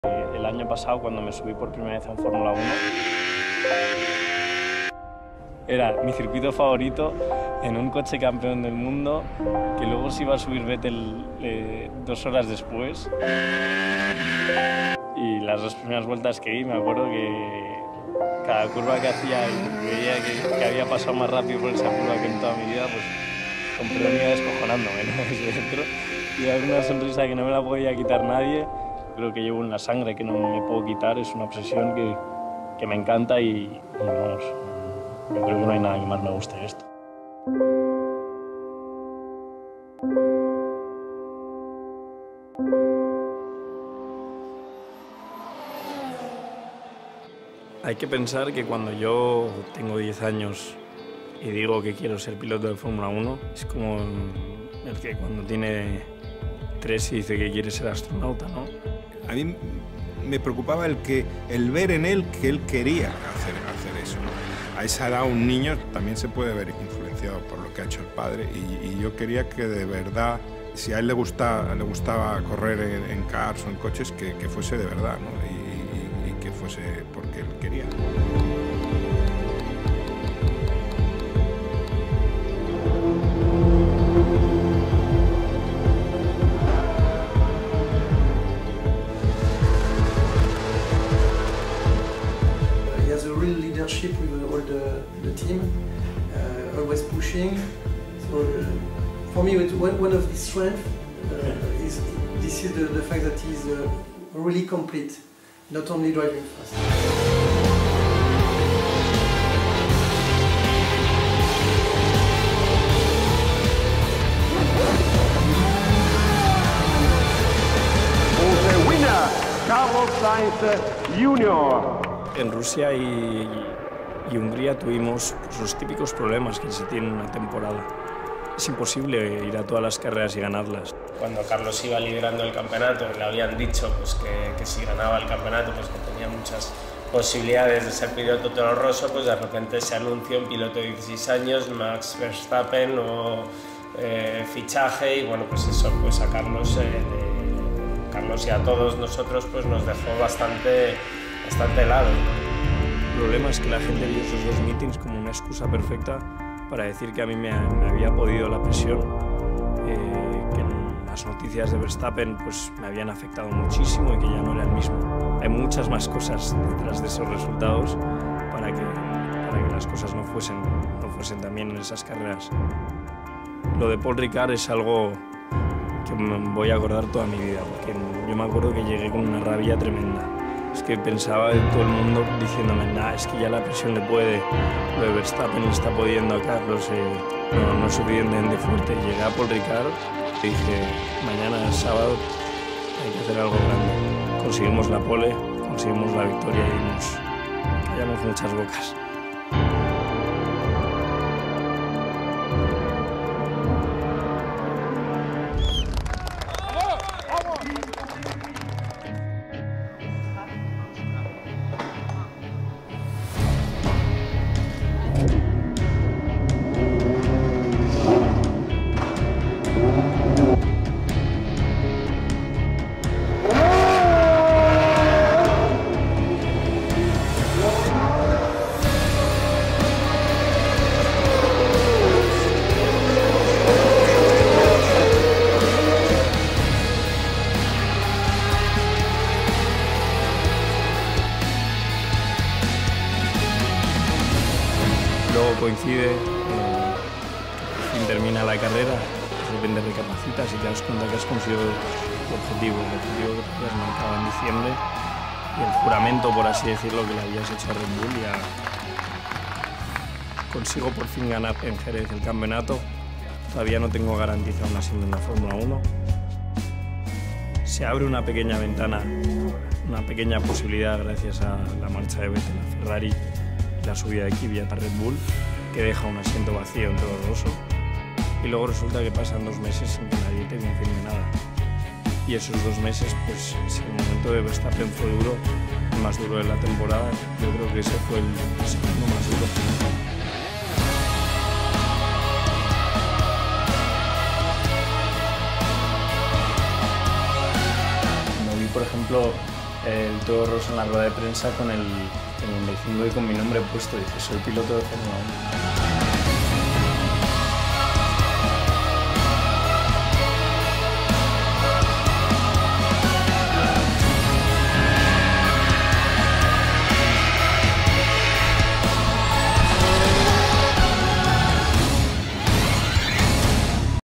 Carlos Sainz journey to F1
He speaks Spanish but I put English subtitles in for you guys